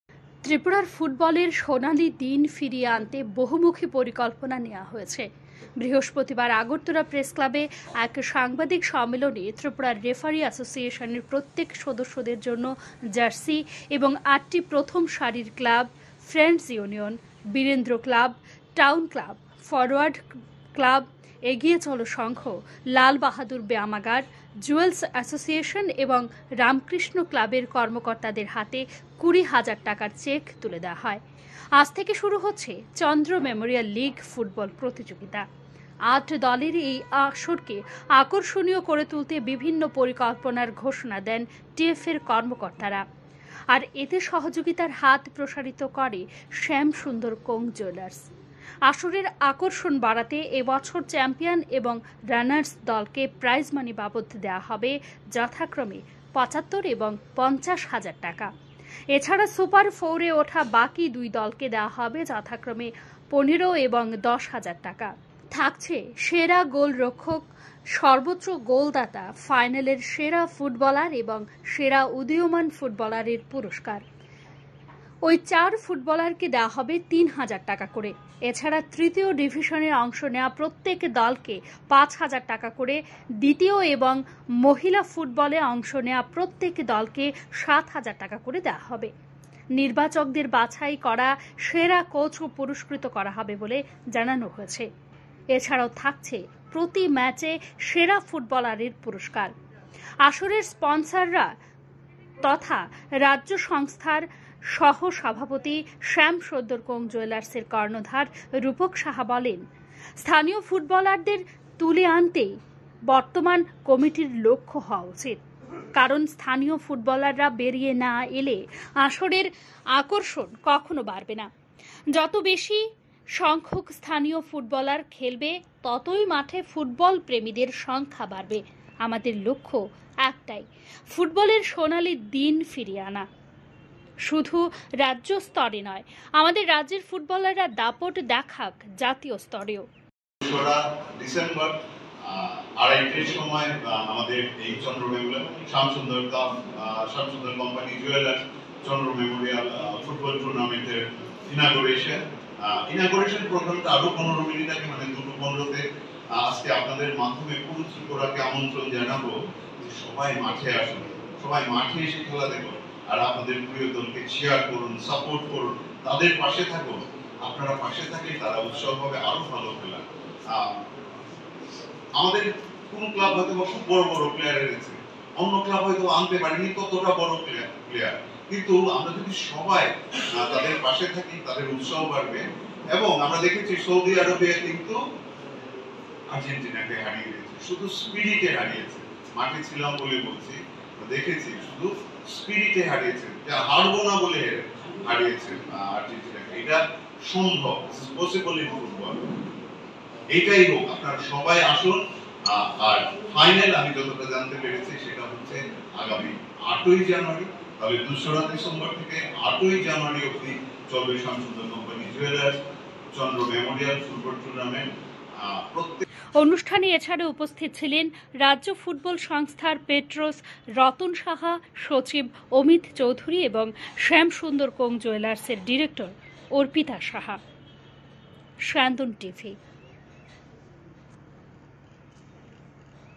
প্রেস ক্লাবে সূচনা হয় শ্যাম সুন্দর কোং জুয়েলার্সের চন্দ্র মেমোরিয়াল ফুটবল আসরের।
আগরতলা প্রেস ক্লাবে এক অনুষ্ঠানের মধ্য দিয়ে সূচনা হয় শ্যাম সুন্দর কোং জুয়েলার্সের চন্দ্র মেমোরিয়াল ফুটবল আসরের।